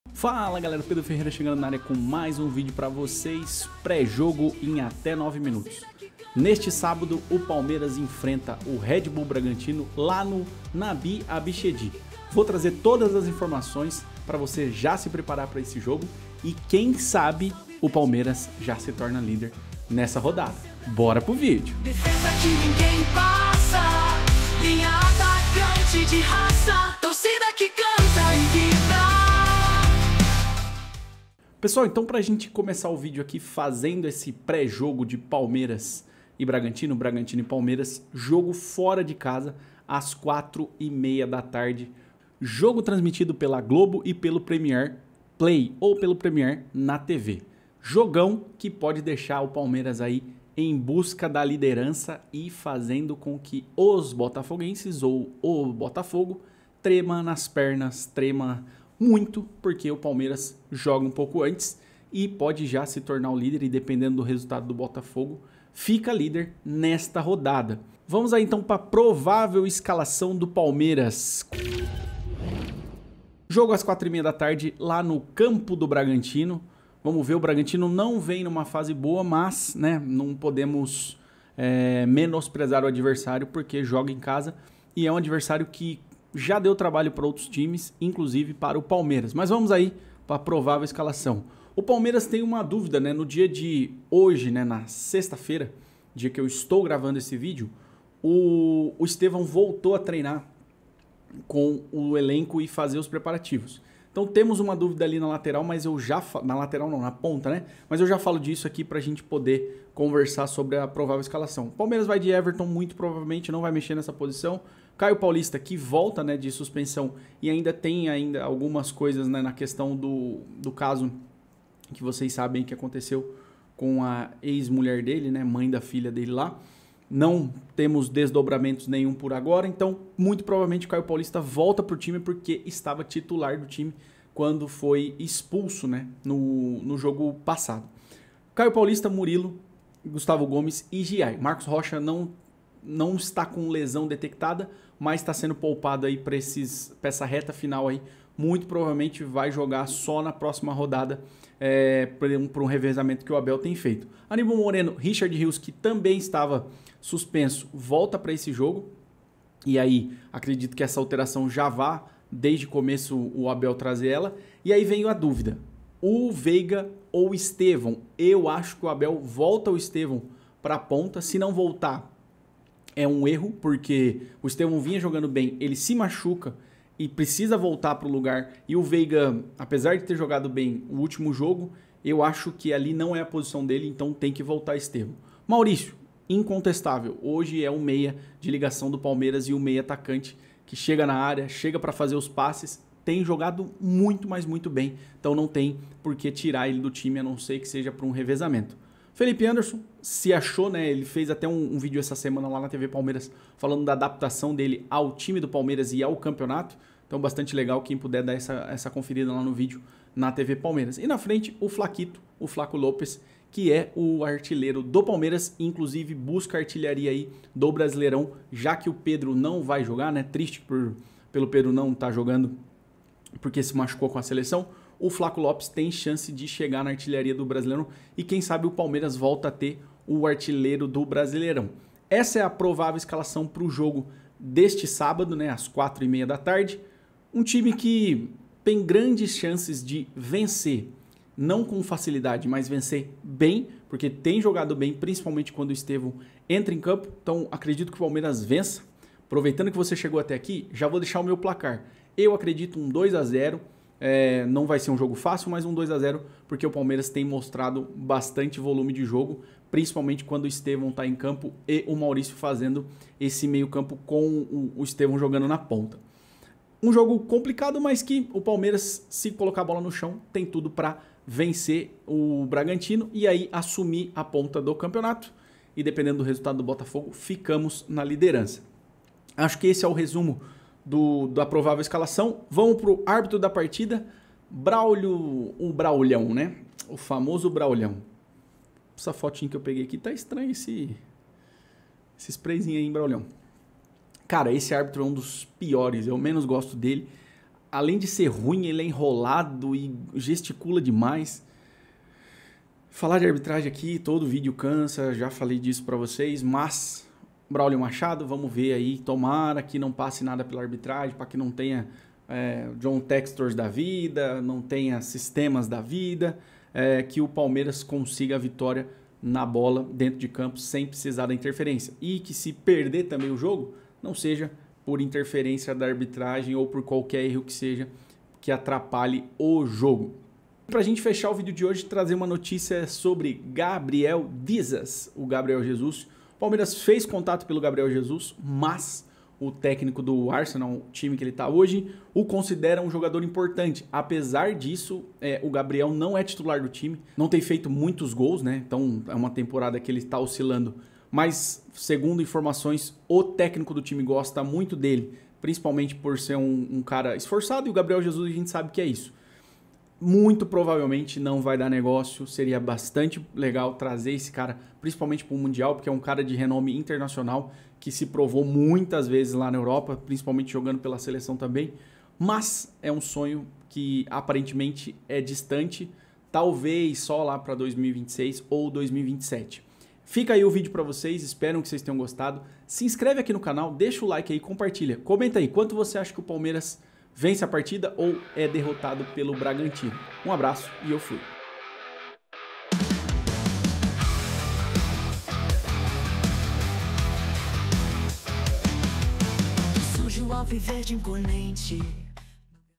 PALMEIRAS A UM PASSO DA LIDERANÇA! MAS SERÁ QUE O RED BULL PODE ESTRAGAR TUDO?
⚽ Palmeiras enfrenta o Red Bull Bragantino em um duelo que pode definir a liderança do Brasileirão de vez! O Verdão vem embalado e busca tomar a ponta de forma definitiva. Será que esse é o jogo que vai colocar o Palmeiras no topo para não sair mais? Todos os detalhes da escalação, análise do adversário e expectativas para esse confronto decisivo.

💬 Deixe seu palpite! Palmeiras garante a liderança ou vai ter surpresa? Não se esqueça de se inscrever e acompanhar tudo sobre essa reta final emocionante do campeonato! 💚👊